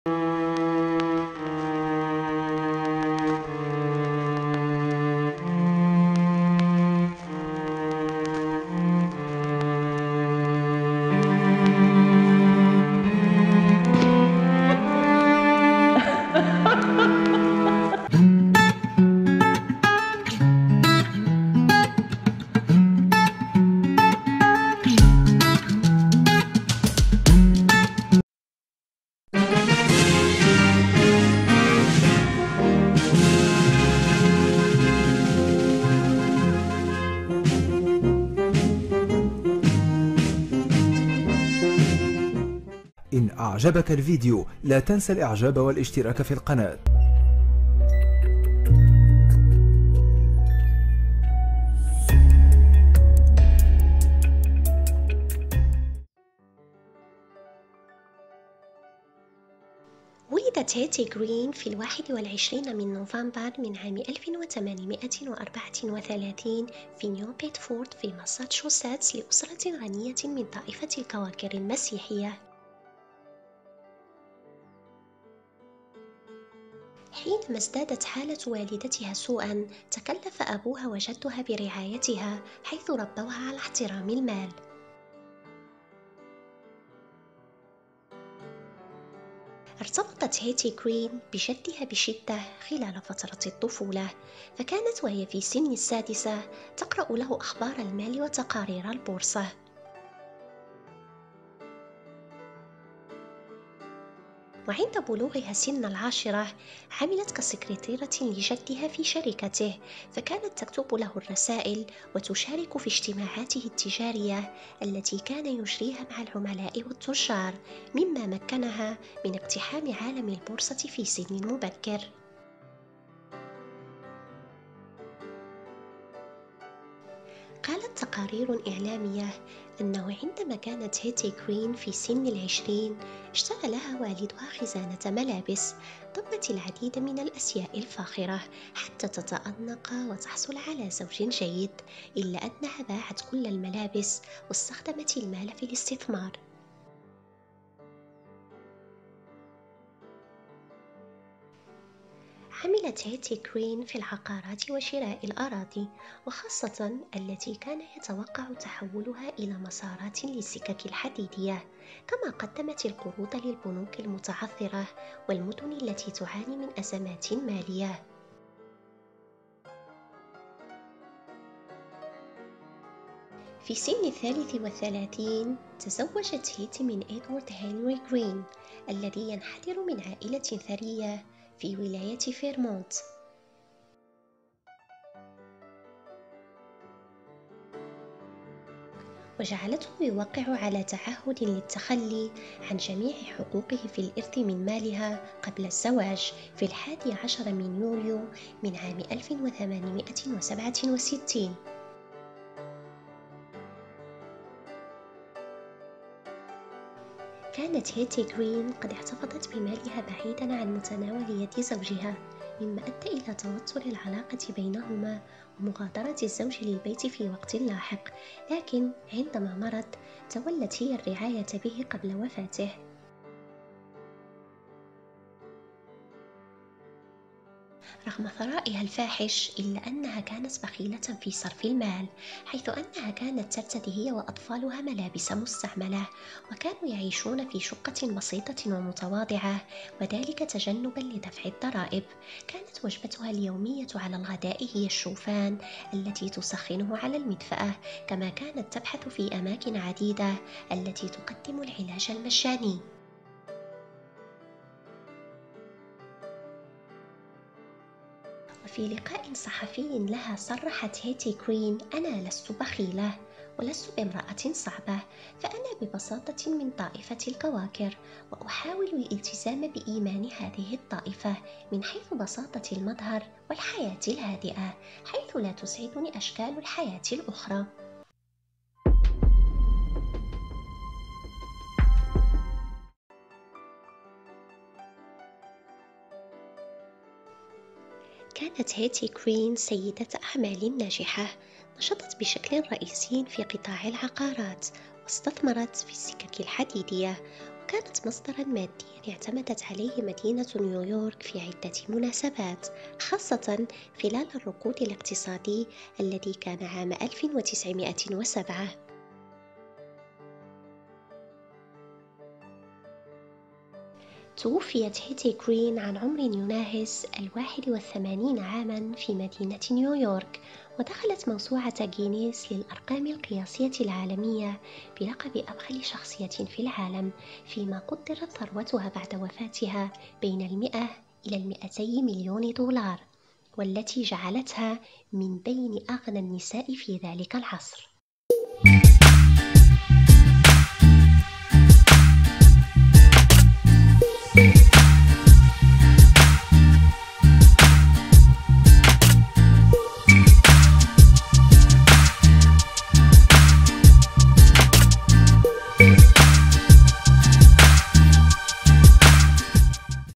الحلقة 4 : هيتي غرين || المرأة الأكثر ثراءا والأكثر بخلا في العالم
سلام الله عليكم أحبابي  أتمنى أن تكونوا بأفضل حال

يتجدد لقاءنا مع حلقة جديدة من سلسلة "قصص بسيناريوهات مختلفة" حيث سنتعرف سويا على قصة  امرأة دخلت موسوعة غينيس للأرقام القياسية العالمية بلقب "أبخل شخصية في تاريخ البشرية " رغم ثرائها الفاحش الذي كان يقدر بين 100 الى 200 مليون دولار مما جعلها من بين أغنى النساء في ذلك الوقت .

إذا كنتم تشاهدونني لأول مرة فلا تنسوا الإشتراك في القناة وتفعيل جرس  الإشعارات ليصلكم كل جديدي .